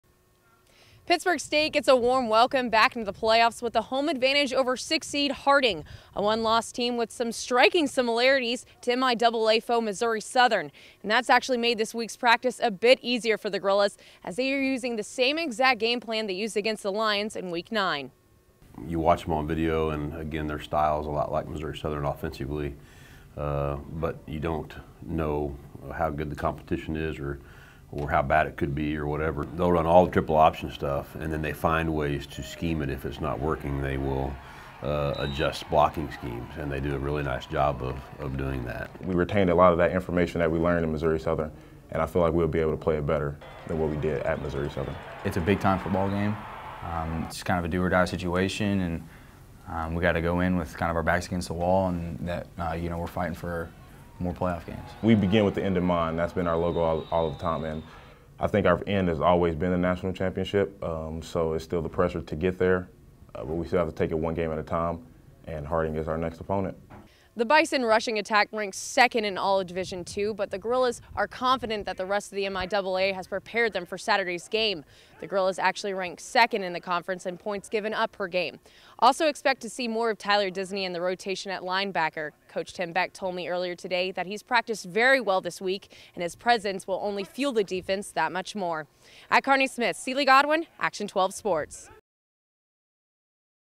Pittsburg State preps for their return to the playoffs
The Gorillas haven't made a return trip to the first round of playoffs since their 2011 national championship run. But after 3 years, PSU plans to come back with vengeance...and it all starts with Harding.